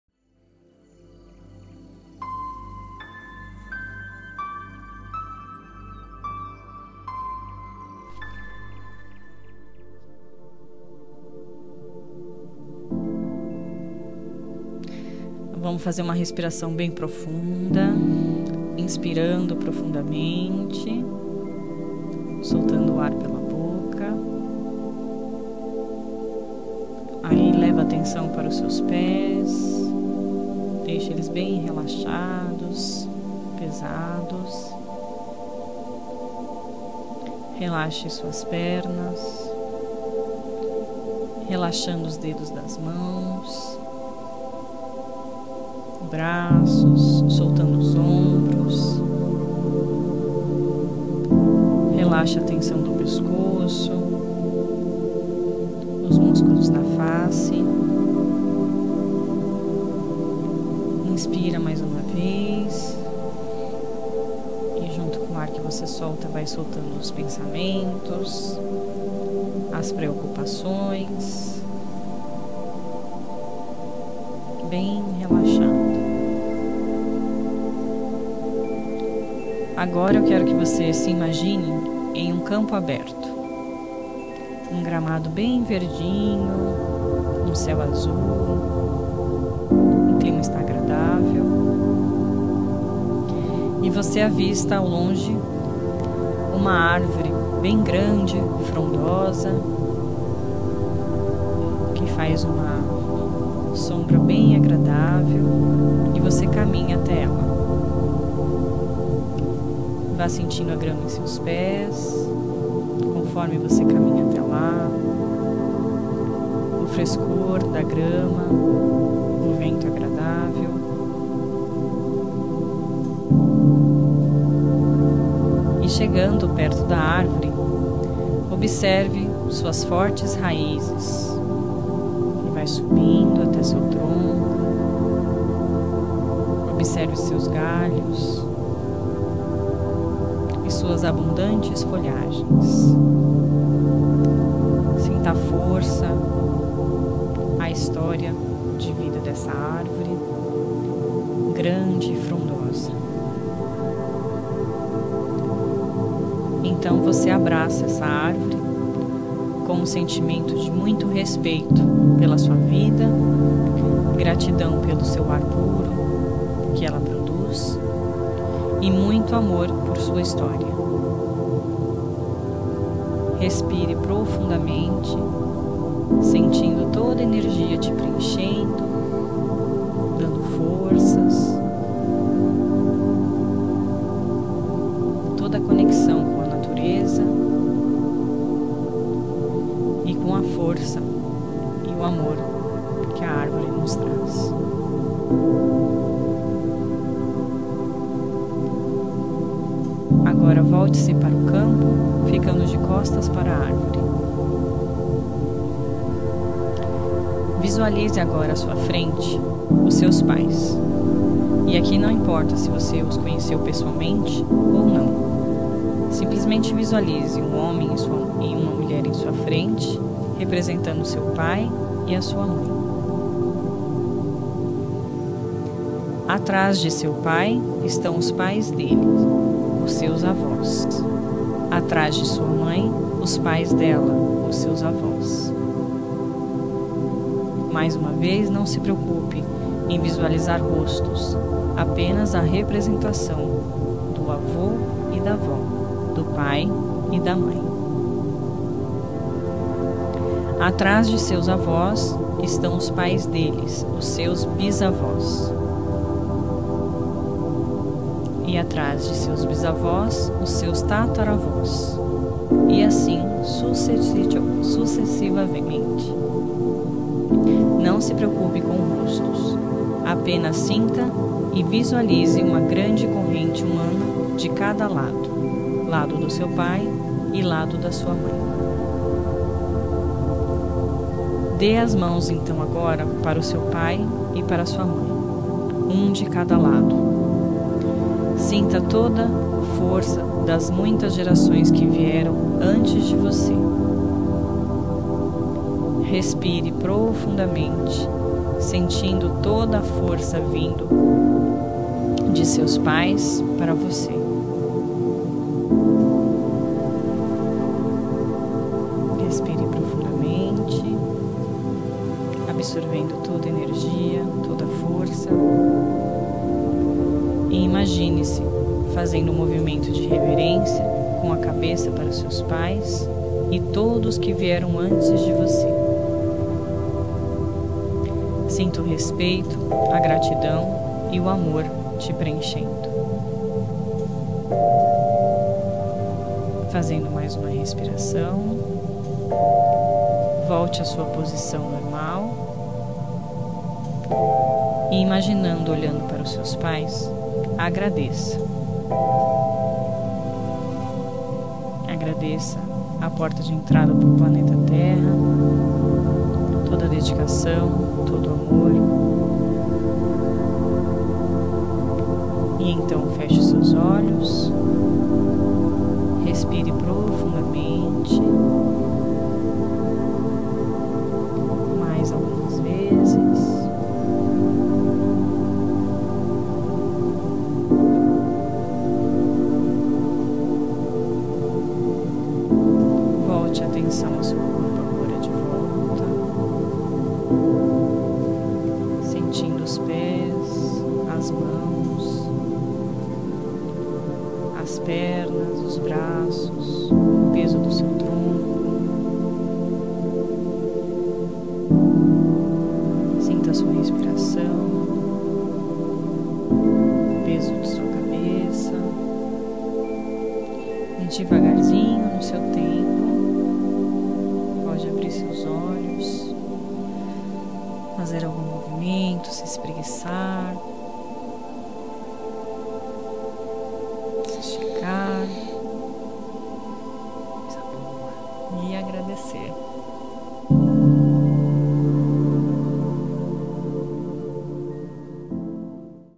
MEDITAÇÃO GUIADA: Meditação Consteladora baseada na Constelação Familiar
Reconecte-se com seus ancestrais e liberte-se de padrões repetitivos, nessa meditação guiada, preparada com base na minha vivência e experiência com a Constelação Familiar, você irá honrar suas raízes e encontrar a força necessária para florescer.

Explore os princípios da Constelação Familiar de forma acessível e acolhedora, de forma suave e calma, como todas as meditações guiadas aqui do canal Relaxar e Meditar.

Desperte a compaixão e o amor incondicional por sua família e dissolva bloqueios emocionais e abra espaço para a cura.

Perfeita para quem busca harmonia nas relações familiares. e deseja compreender seus padrões e crenças limitantes, além de buscar a cura emocional e o autoconhecimento.

♥ Gostou deste vídeo? Comente abaixo e CURTA para colaborar com o nosso trabalho, sua opinião é muito importante.